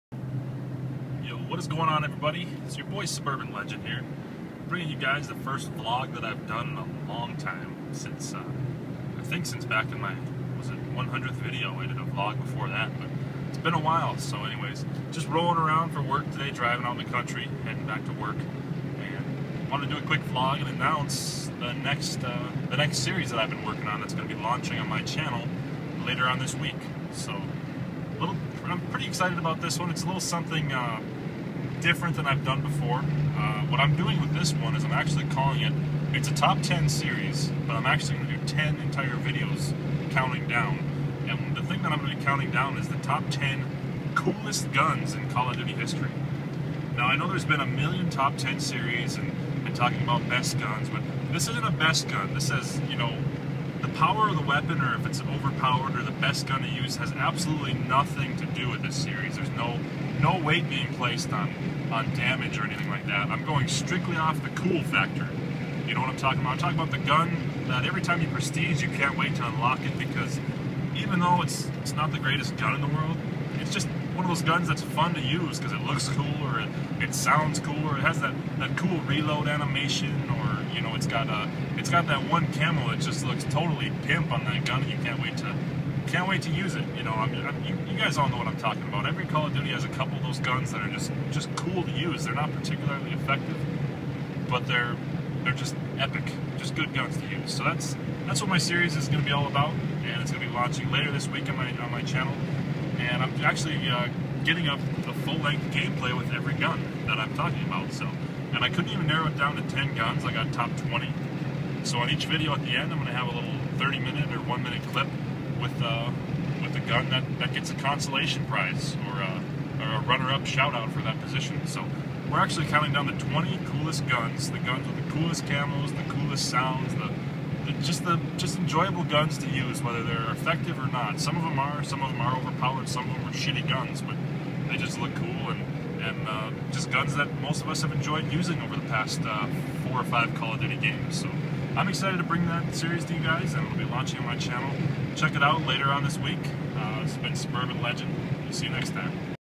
New Series Announcement! Vlog
New series launching this week. The 10 Coolest Guns in COD History! 10 full length gameplay commentaries counting down to the coolest gun of all time!